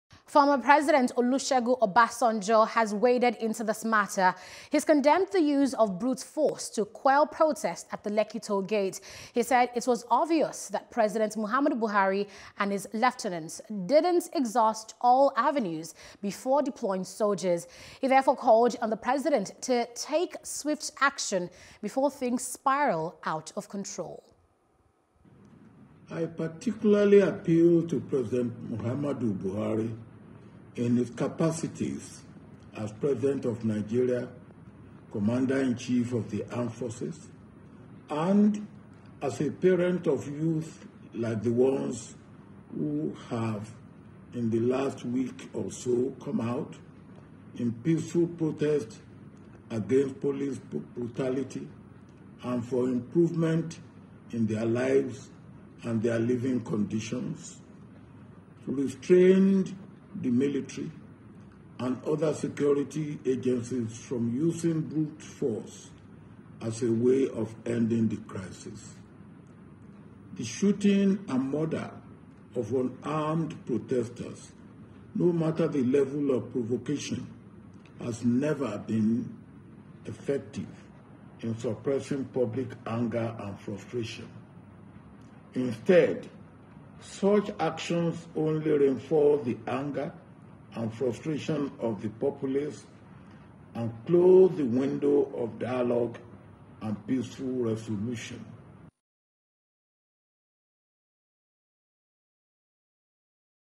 ENDSARS: “Killing Protesters Worsens Crisis” - Obasanjo Tells Buhari (NEWS | NIGERIA)
“Killing Protesters Worsens Crisis” - Obasanjo Tells Buhari